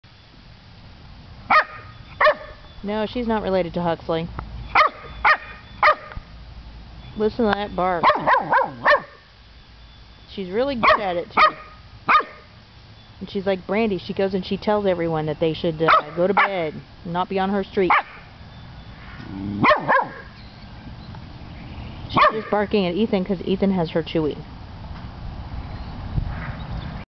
Savannah Barks
Savannah shows her voice and her similarity to some relatives